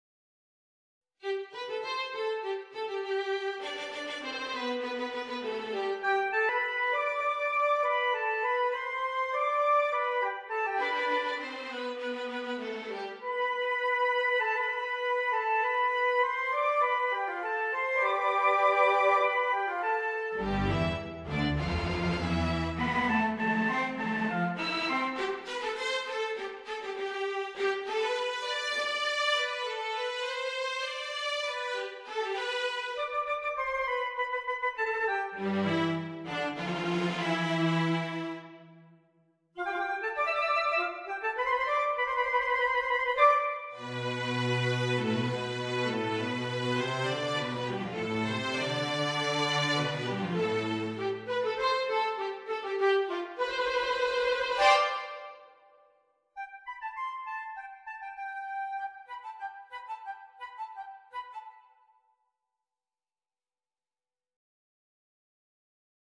Trill and Tremolo tunes
South Indian tunes I composed using triil and Tremolo technics